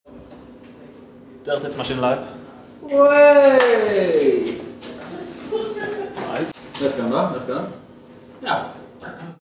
Tidsmaskinen.no er LIVE!
Fredag 16. januar kl 10.00 ble åpnet for publikum. Slik så det ut hos Appex :-)